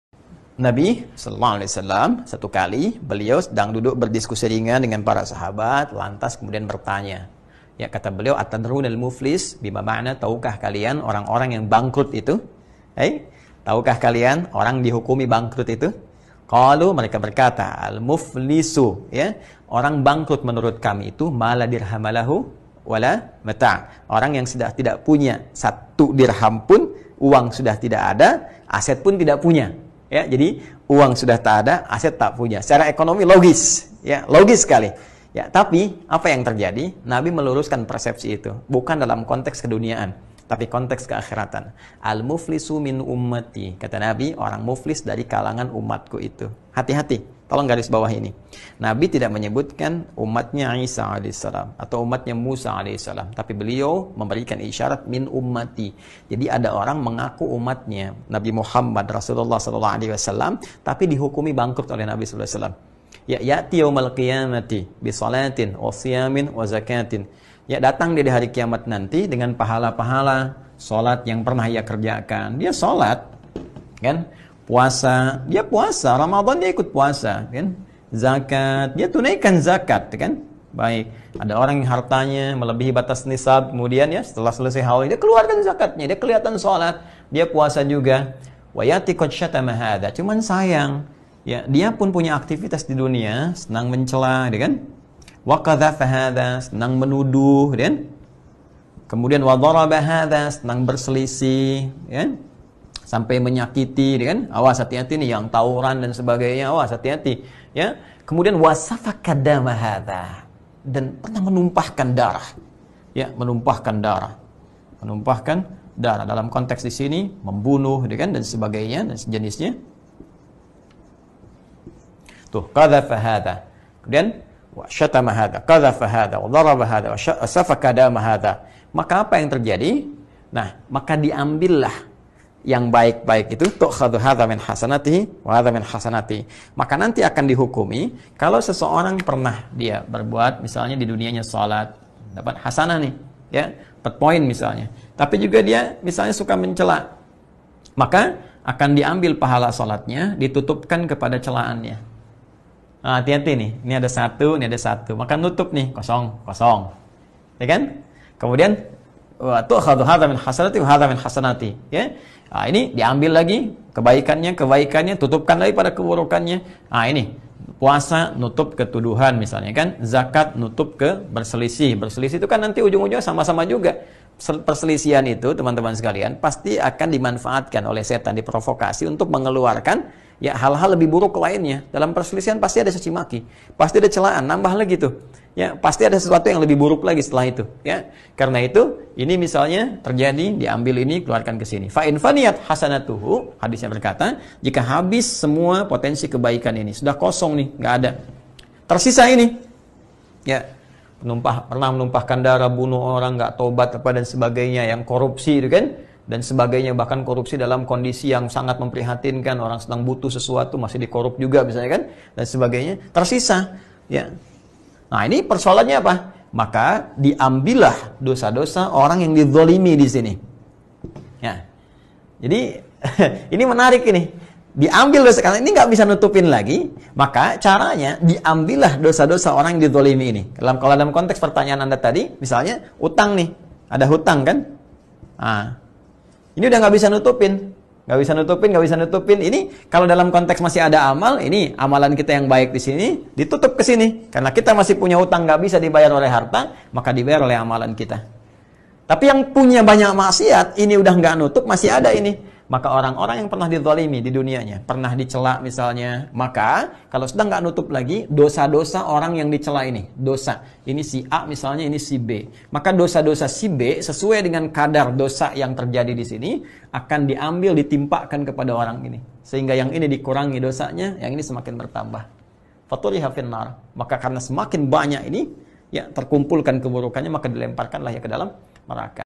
Orang Bangkrut
Beberapa cuplikan ceramah dari Ust Adi Hidayat